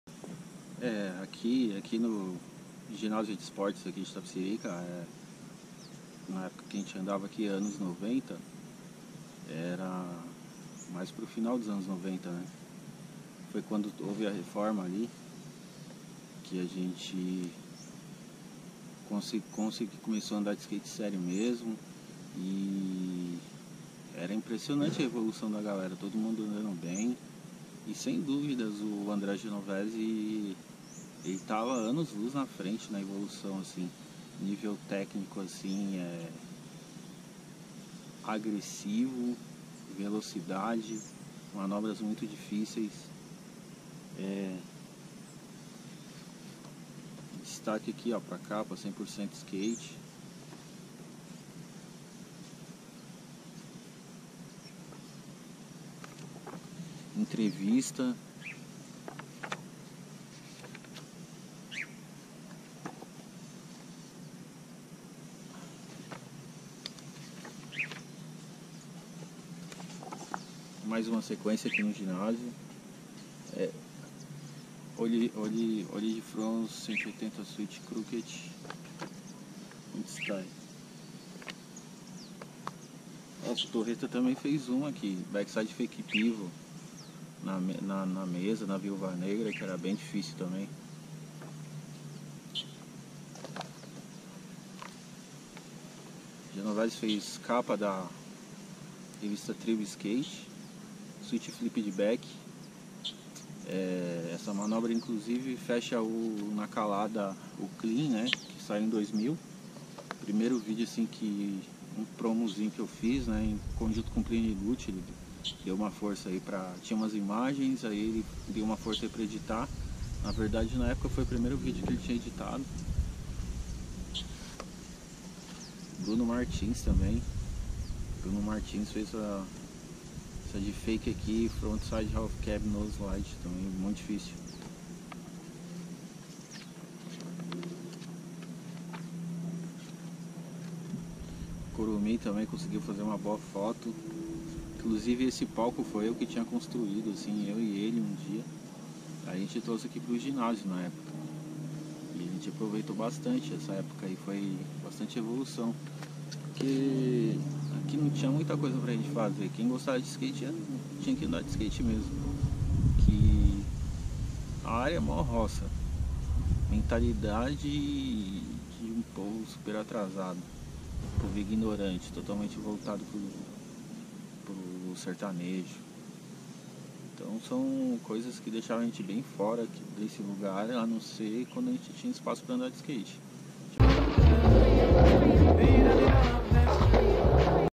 Skate em Itapecerica da Serra #02
skateboarding Quando ouve uma reforma nos arredores ginásio de Itapecerica da Serra em 1997, teve uma evolução muito rápida para os skatistas que foram locais de lá, Jorge Costa contou exatamente como foi essas vivências!!